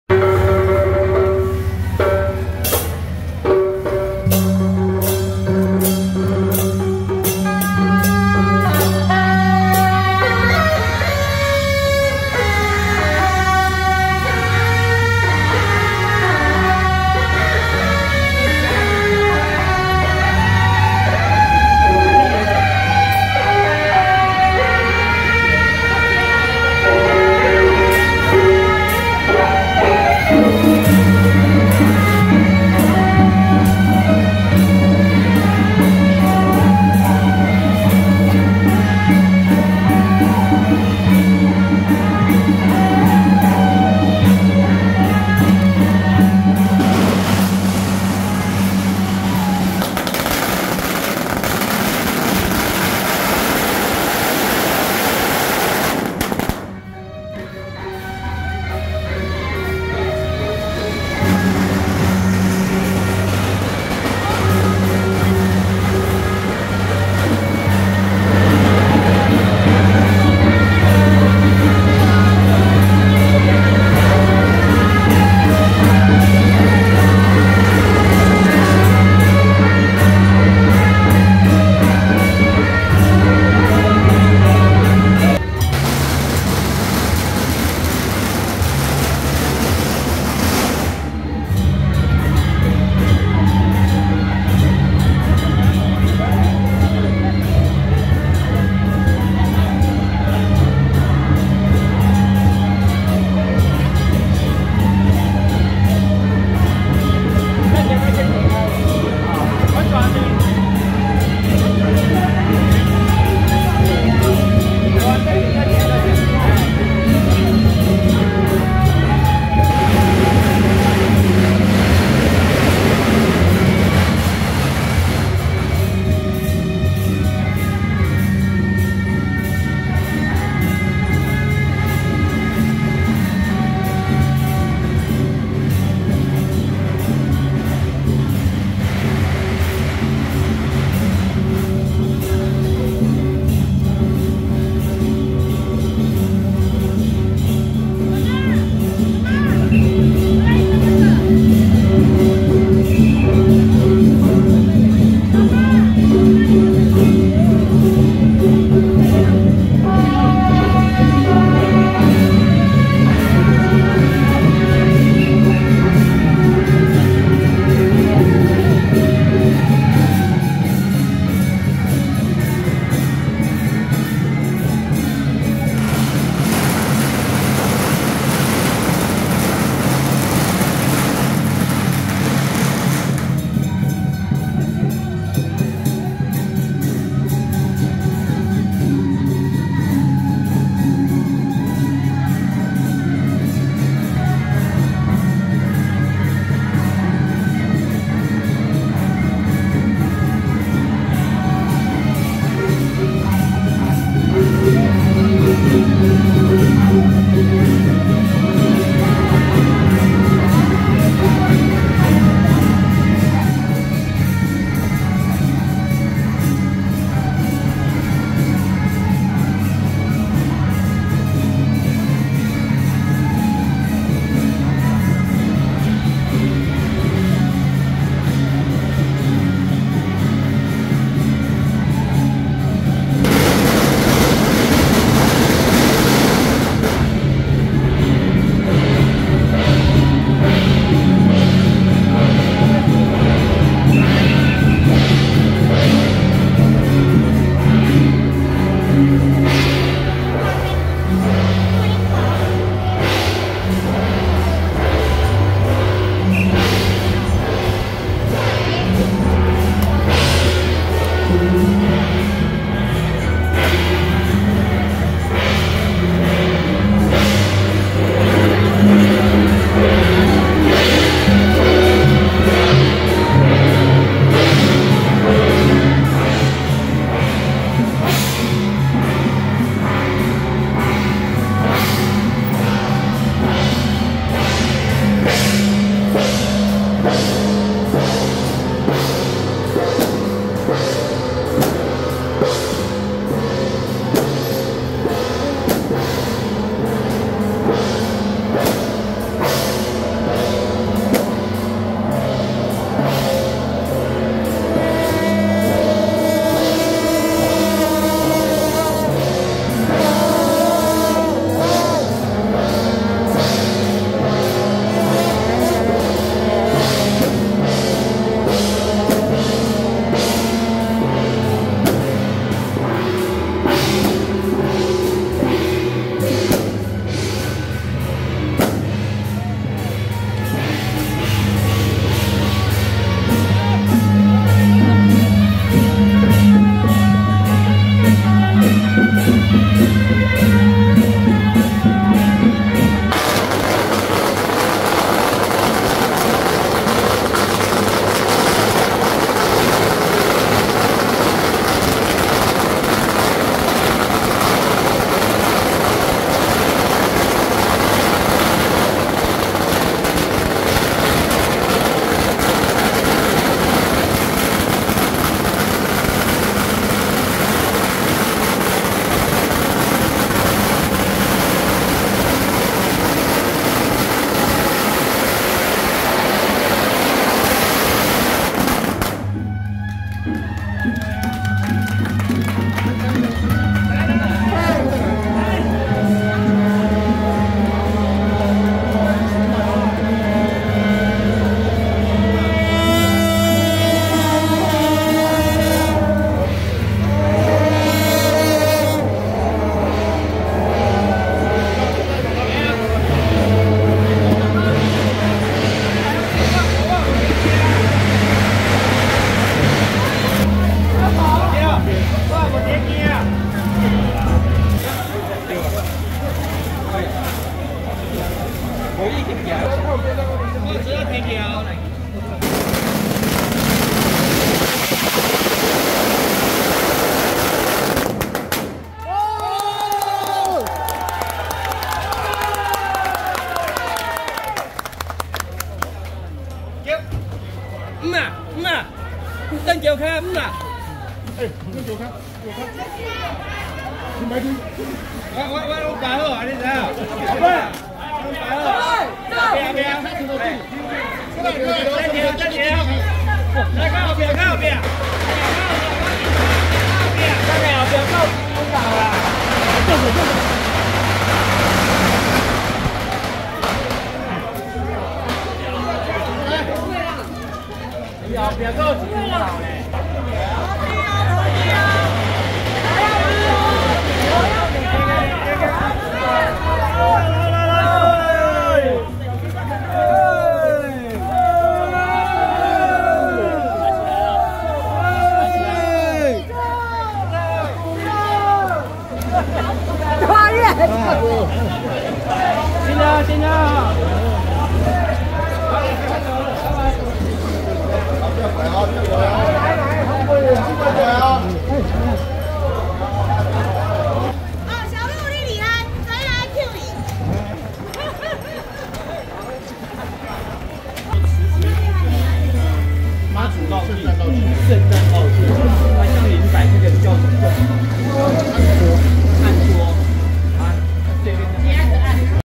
【士林慈諴宮-神將團＆天上聖母(士林媽) 聖駕(迴龍鎮安壇轎班團)】~113士林慈諴宮恭祝天上聖母聖誕遶境
士林湳雅永安社-北管＆哨角團＆士林慈諴宮-神將團＆天上聖母 聖駕